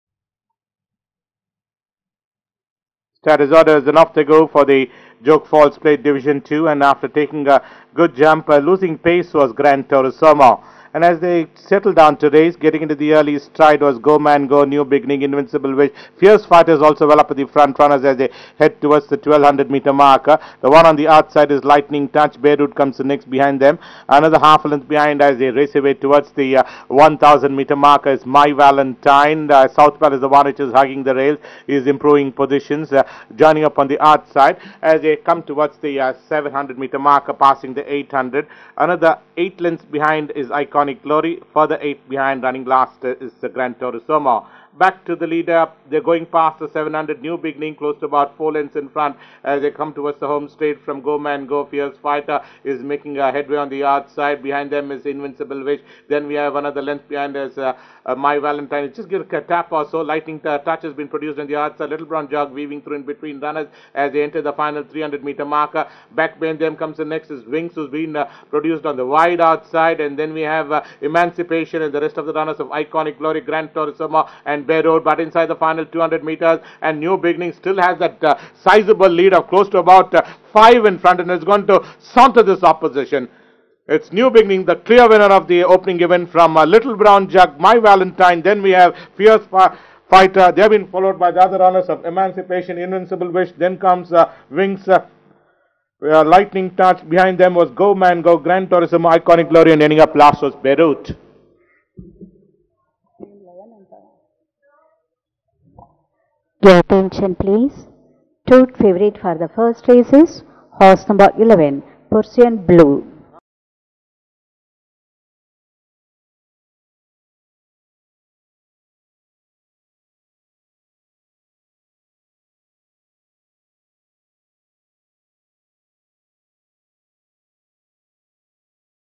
New Beginning with P P Dhebe up wins The Jog Falls Plate Div 2 2018
New Beginning trained by Ranjeet Shinde with P P Dhebe up wins The Jog Falls Plate Div 2 2018 at The Mysore Race Club